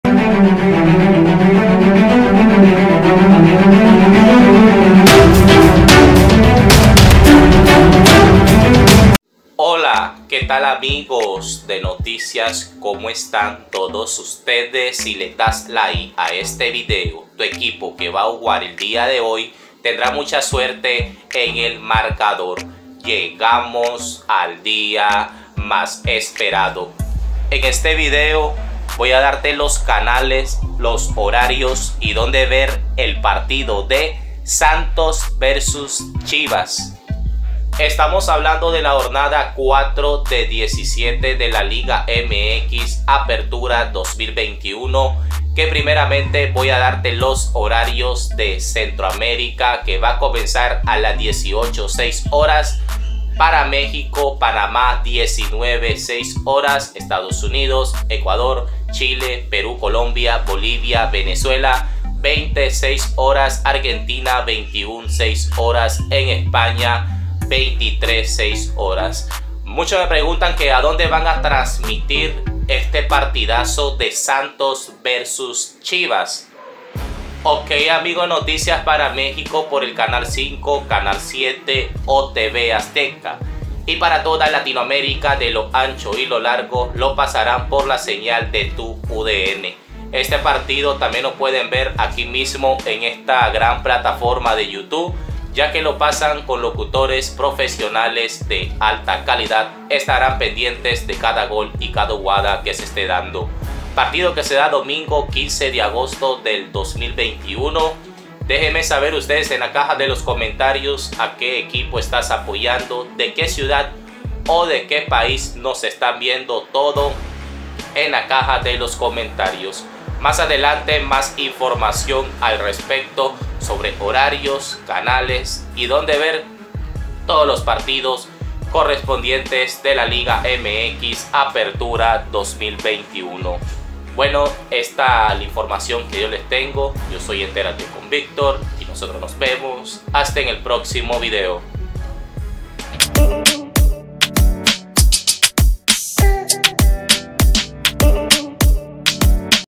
Donde ver Santos vs. Chivas en vivo, por la Jornada 4, Liga MX 2021
Ver Santos vs. Chivas, Donde ver, en que canal ver, hora, día, en vivo, Chivas vs. Santos Domingo 15 de Agosto 2021, les dejo los canales con país y donde ver hoy el partido Santos Laguna vs. Chivas Jornada 4 de 17 Liga MX, Apertura 2021, desde el estadio TSM Corona.